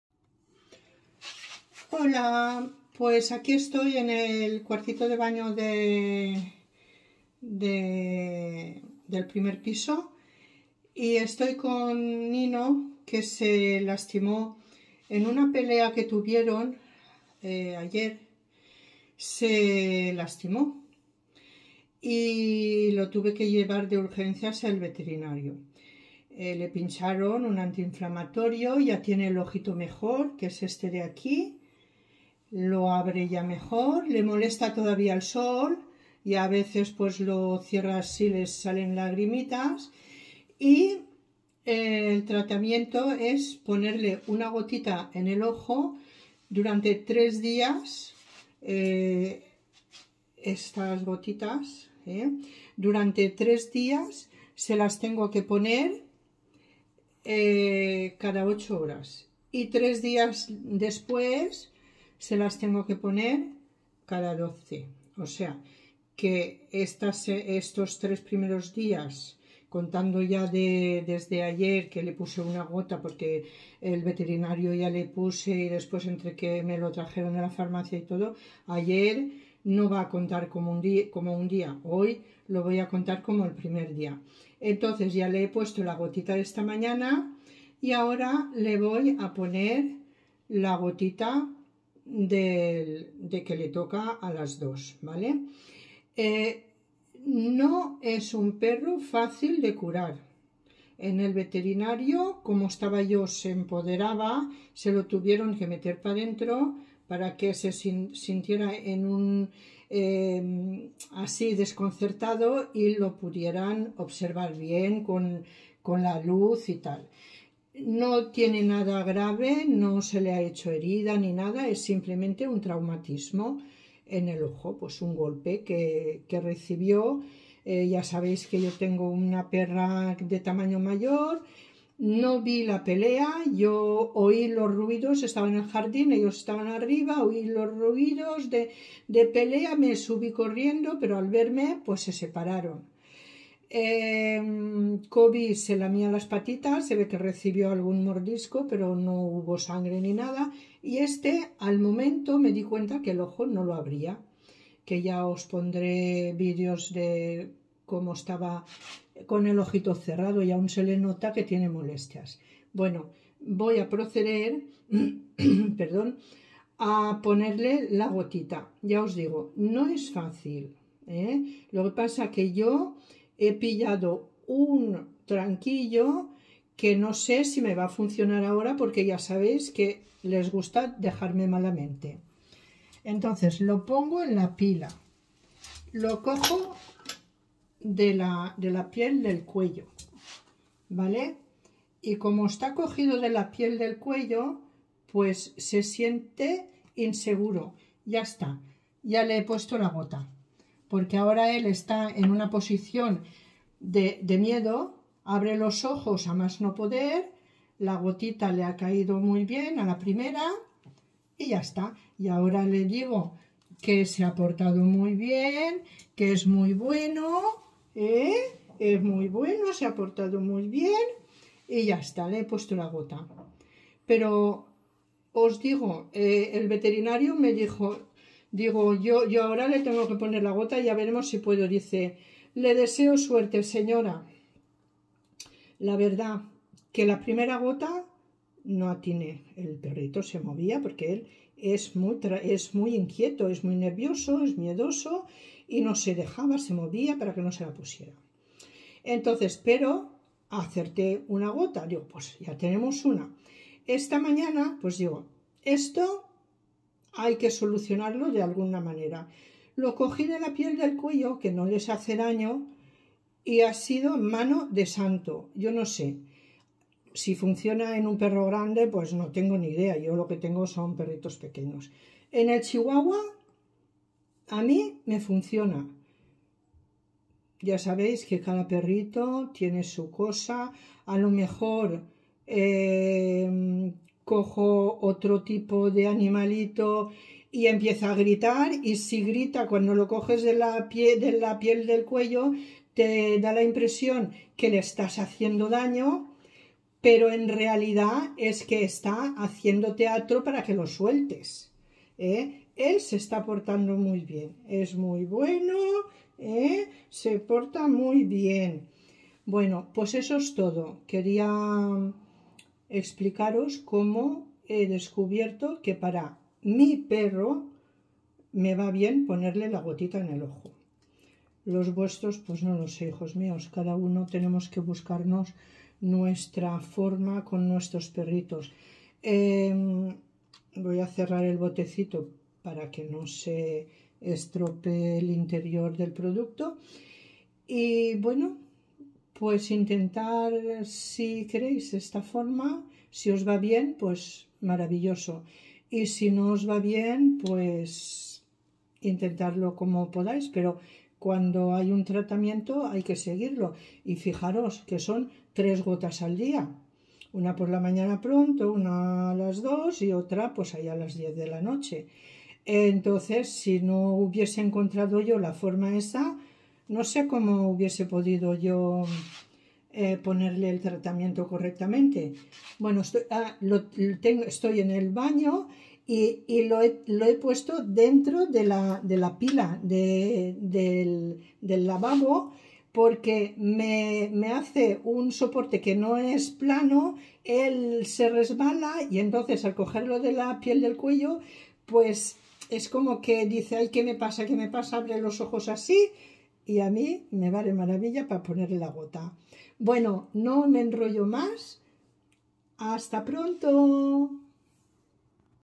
GOTA facil en OJO de CHIHUAHUA mundos chihuahuas tip gota en ojo
EXPLICO como pongo gotas en los ojos de mis chihuahuas. @mundoschihuahuas